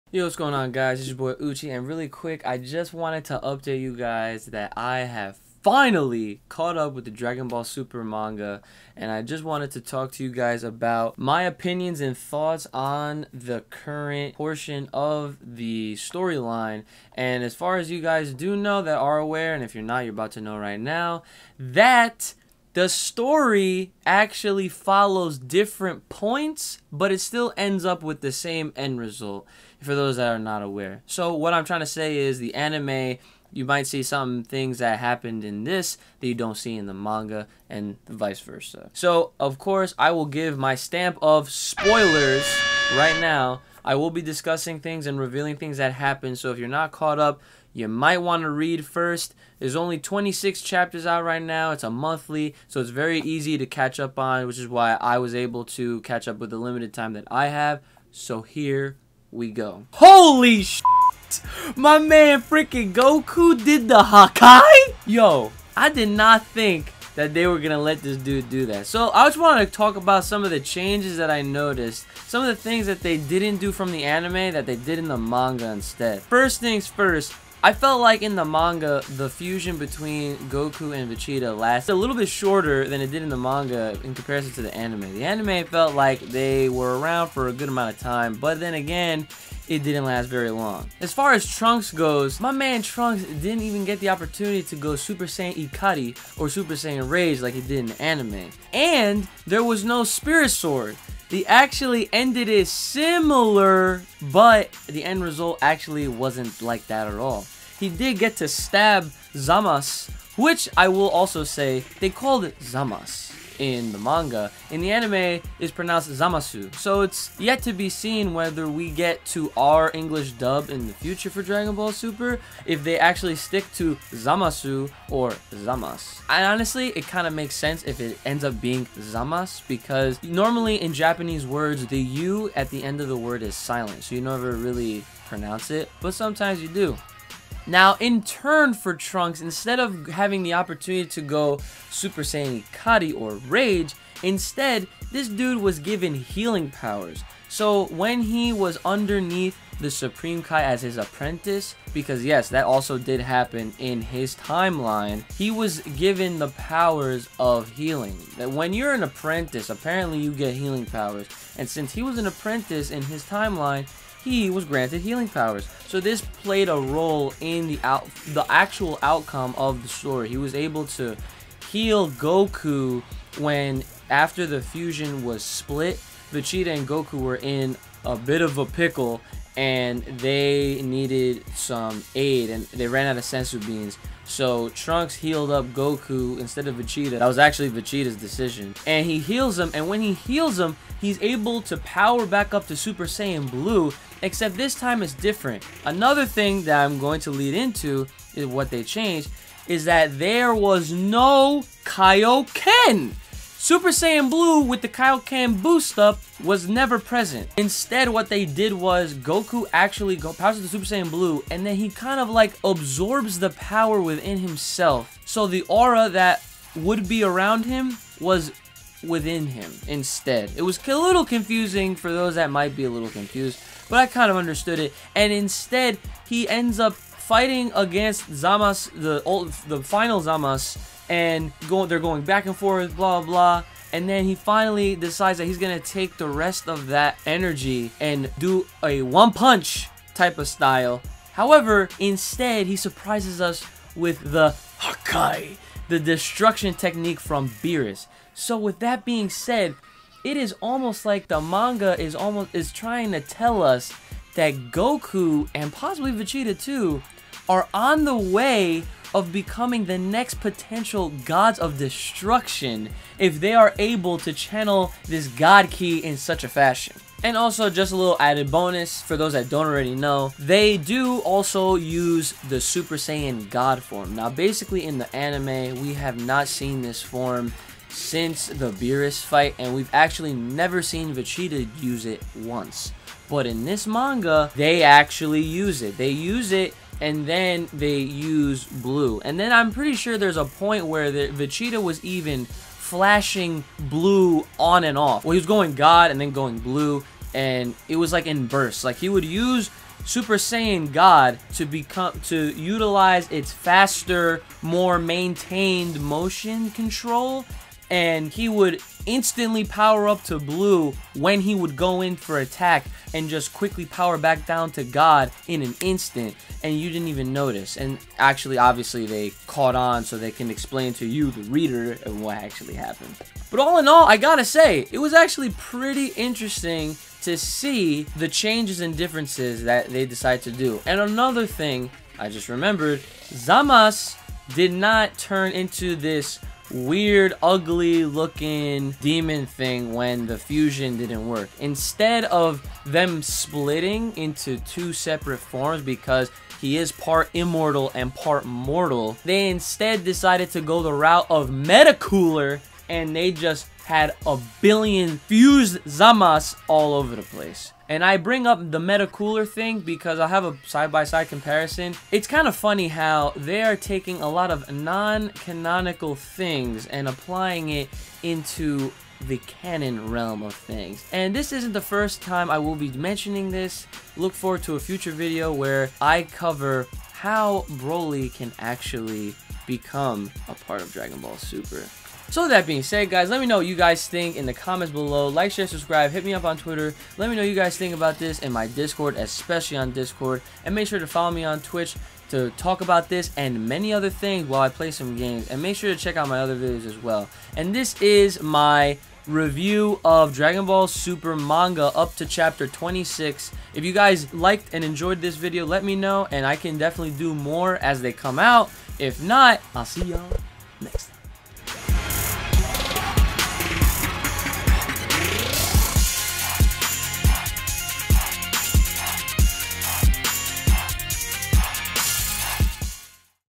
Dragon Ball Super Manga Update | Anime Differences | My Thoughts up to Ch.26
Finally caught up with the manga! I go over some of the differences i noticed in comparison to the Anime.

Follow HouseTop!

Follow Tempestt!